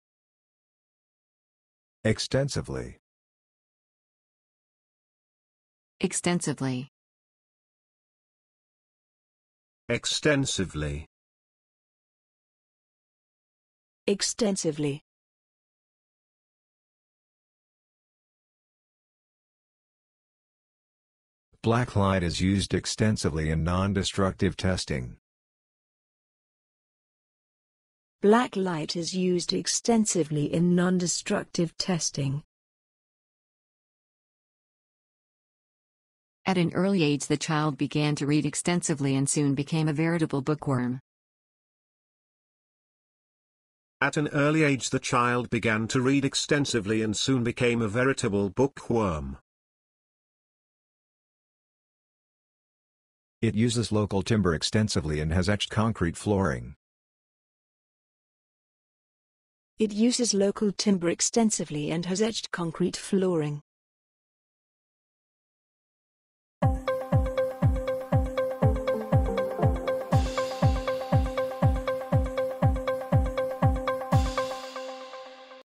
EXTENSIVELY pronunciation | Improve your language with bab.la
Improve your spoken English by listening to EXTENSIVELY pronounced by different speakers – and in example sentences too. Learn and love languages with bab.la.